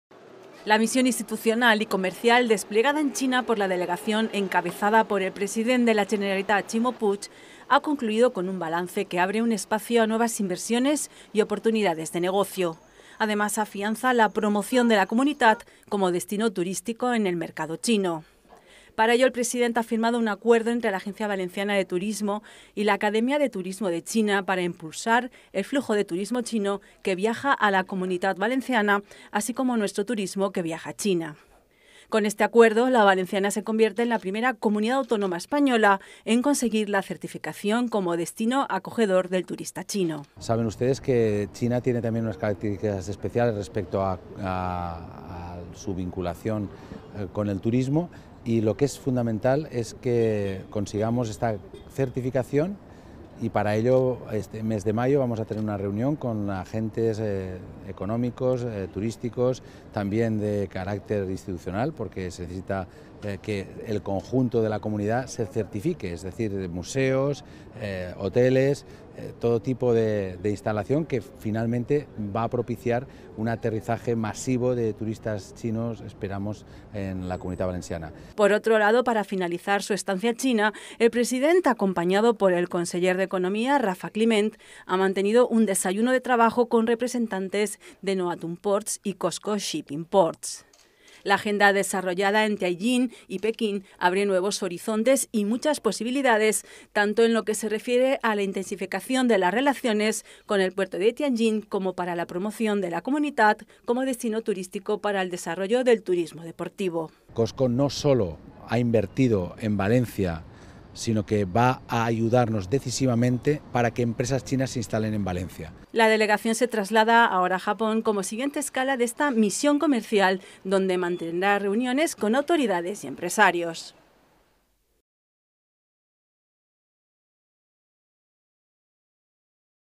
La misión de la GVA refuerza las relaciones con China y abre espacio para nuevas oportunidades
La misión institucional y comercial desplegada en China por la delegación encabezada por el president de la Generalitat, Ximo Puig, ha concluido este martes con un balance que deja como saldo el fortalecimiento de las relaciones comerciales con este país y que abre espacio a nuevas inversiones y oportunidades de negocio para empresas tanto valencianas como chinas, además de afianzar la promoción de la Comunitat Valenciana como destino turístico en el mercado chino.

La missió institucional i comercial desplegada a Xina per la delegació encapçalada pel president de la Generalitat, Ximo Puig, ha conclòs aquest dimarts amb un balanç que deixa com a saldo l'enfortiment de les relacions comercials amb aquest país i que obri espai a noves inversions i oportunitats de negoci per a empreses tant valencianes com a xineses, a més d'afermar la promoció de la Comunitat Valenciana com a destinació turística en el mercat xinés.